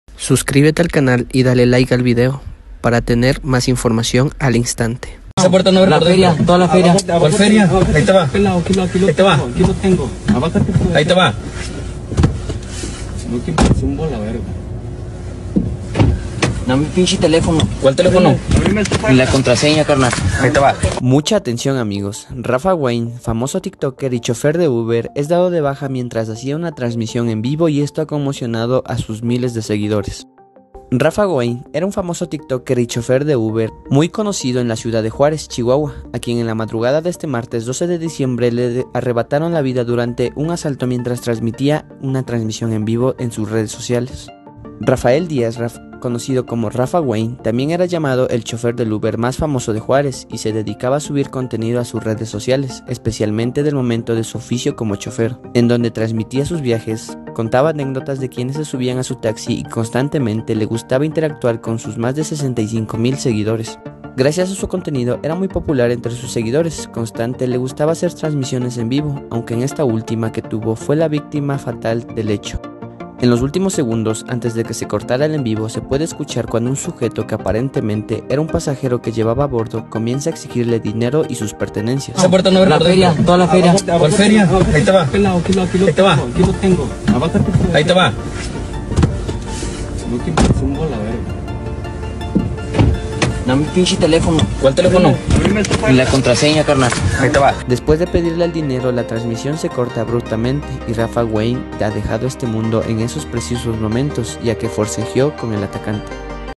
Video del momento exacto que as3sinan a influencer Rafa Wayne conocido como el "Uber tiktoker"
Detalles del as3sinato de Rafa Wayne influencer conocido como uber tiktoker.